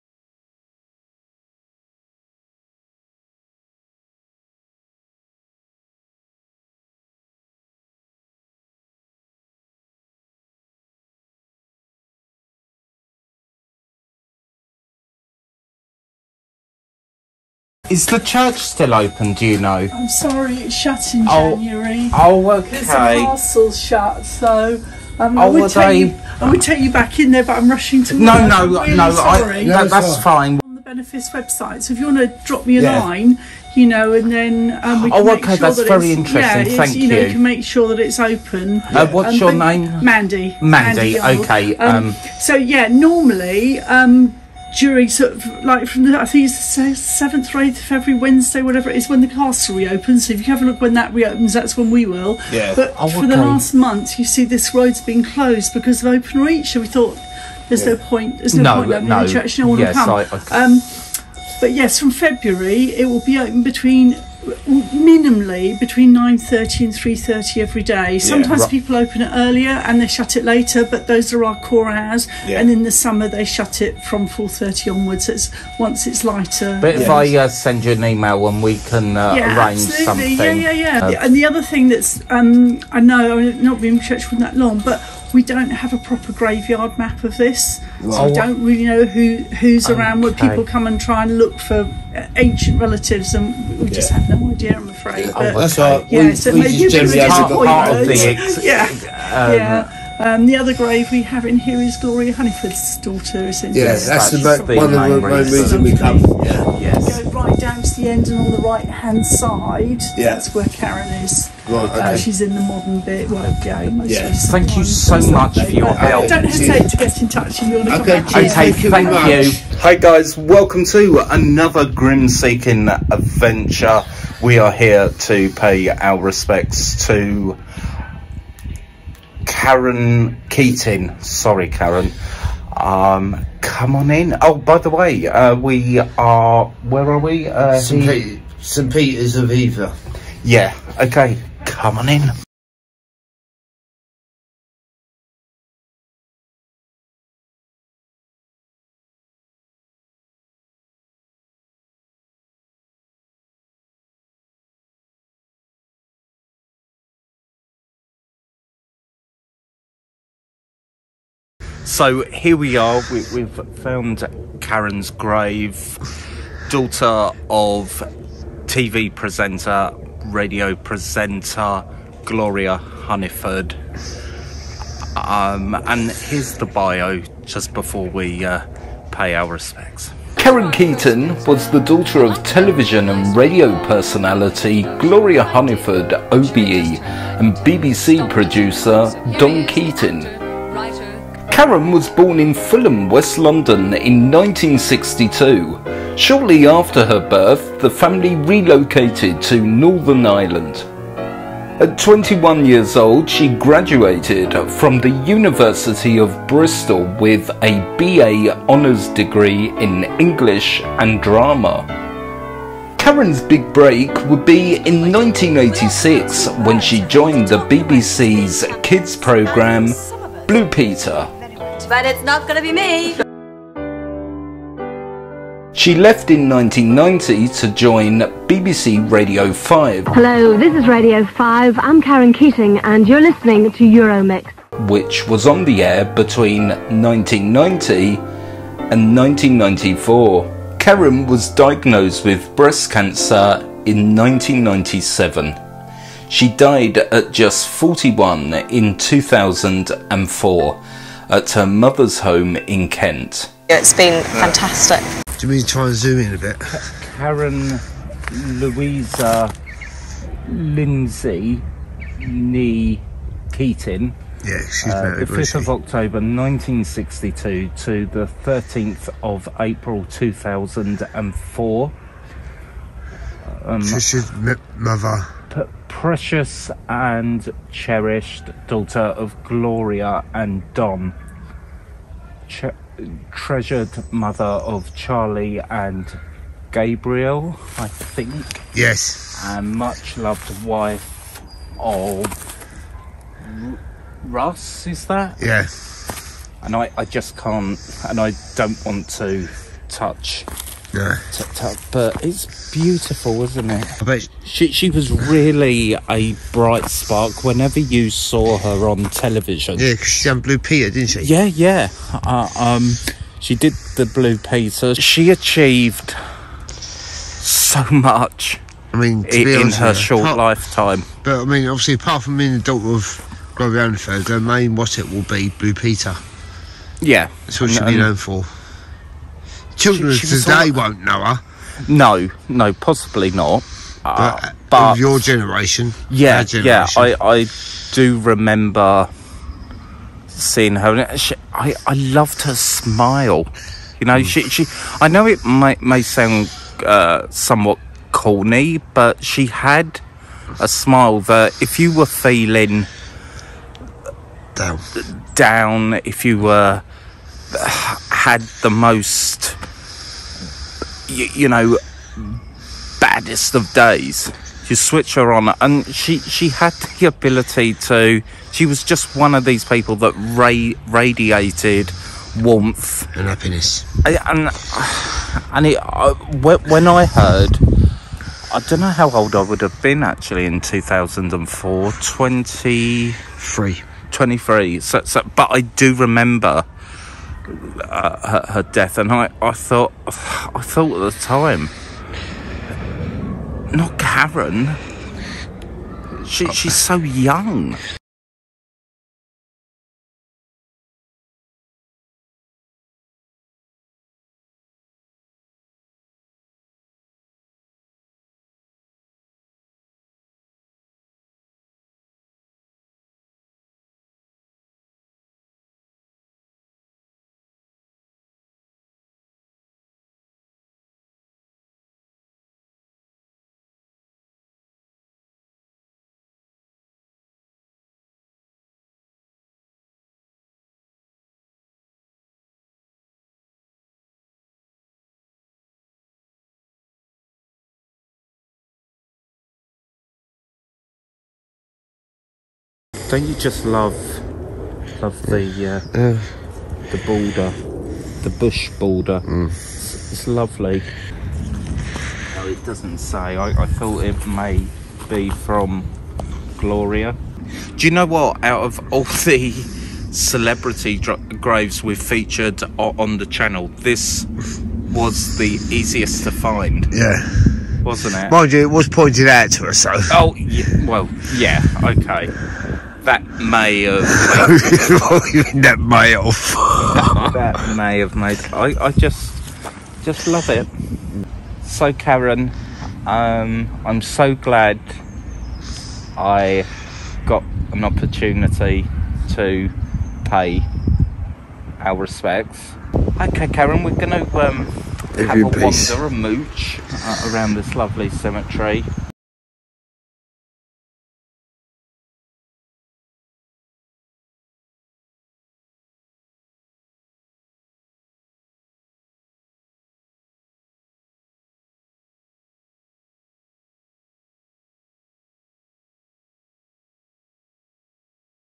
BBC BLUE PETER'S CARON KEATING GRAVE
Join us as we travel to Hever in England to find Carom Keating and pay our tribute.

This is 1 of 3 special uploads this weekend at Hever.